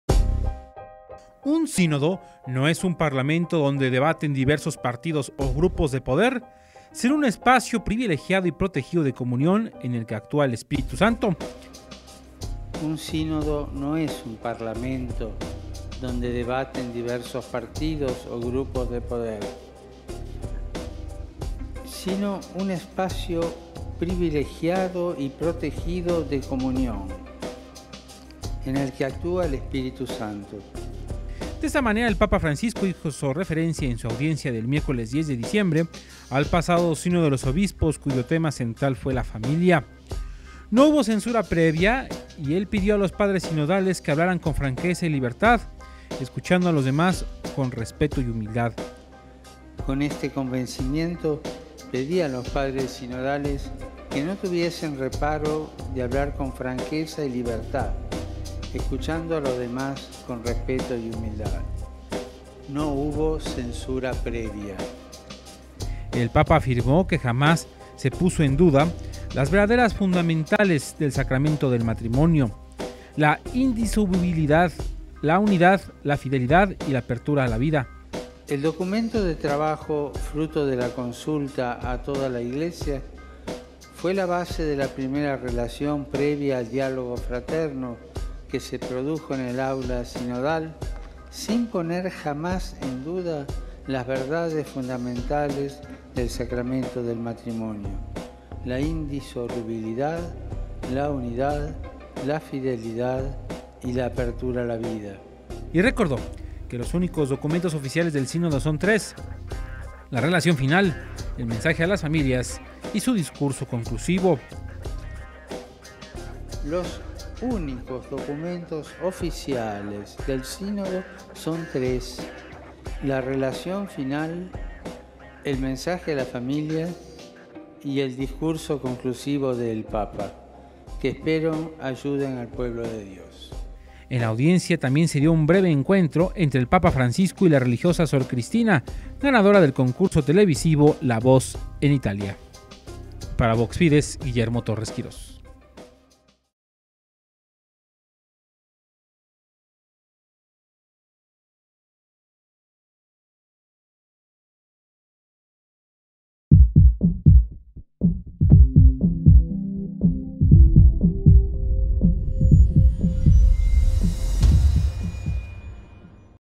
Sínodo: espacio donde actúa el Espíritu Santo, no un Parlamento
Recuerda el Papa que los documentos oficiales del Sínodo de la Familia son: Relación final, Mensaje a las familias y el Discurso conclusivo del Papa.